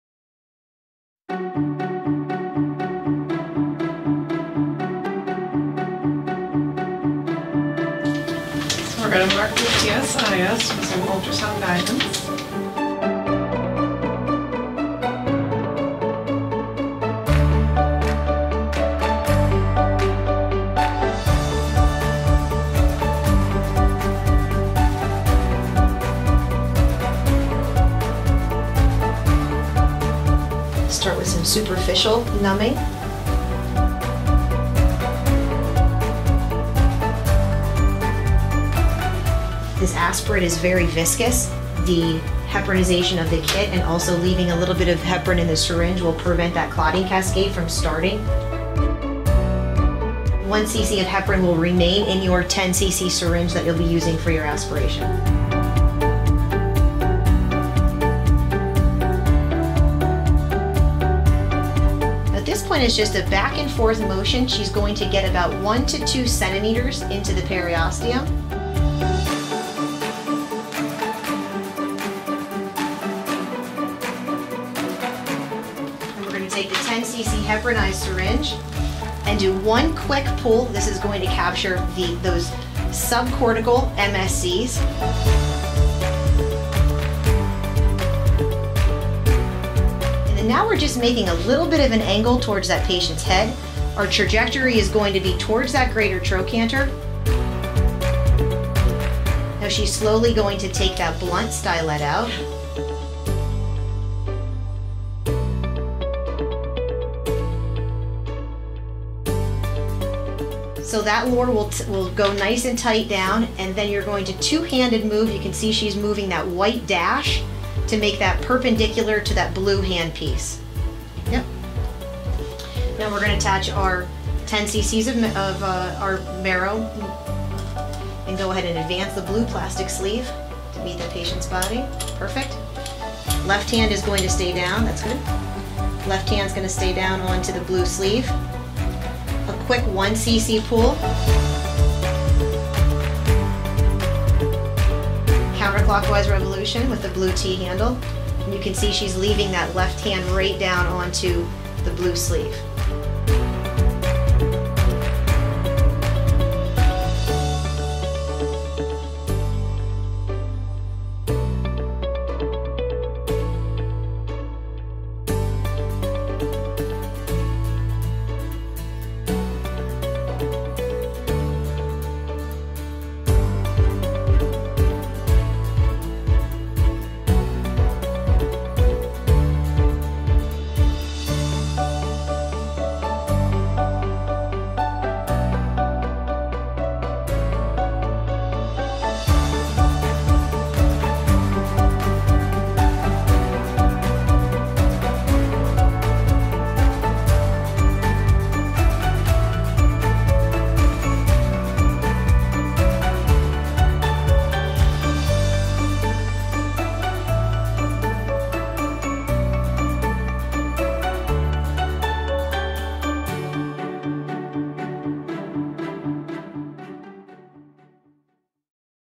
Use of Marrow Cellution in an office setting
Bone marrow aspiration with the MARROW CELLUTION™ MC-RAN-11C System in an office setting.

The Marrow Cellution™ Bone Marrow Aspiration & Cancellous Bone Graft Harvesting System is a novel bone marrow access and retrieval device that incorporates features designed to minimize limitations of traditional trocar needles.

Marrow Cellution's main advantages are:
✔ never leaves the sterile field
✔ no further manipulation required (i.e. centrifugation or lab manipulation)
✔ maximizes cell yield
✔ minimizes blood infiltration
✔ reduces donor site morbidity
✔ minimally invasive
✔ regulatory compliant
✔ percutaneous bone collection

Marrow Cellution™ maximizes stem and progenitor cell recovery and minimizes peripheral blood infiltration.

Marrow Cellution™ accesses aspirate flow collected exclusively laterally as the tip of the aspiration cannula is closed allowing marrow collection perpendicular to and around the channel created by the device. It incorporates technology to precisely reposition the retrieval cannula within the marrow space after each aspiration.
These features achieve a clinicians‘ desire for a single entry point. A single puncture with Marrow Cellution™ provides high quality bone marrow aspirate and bone graft, collected from numerous sites within the marrow geography.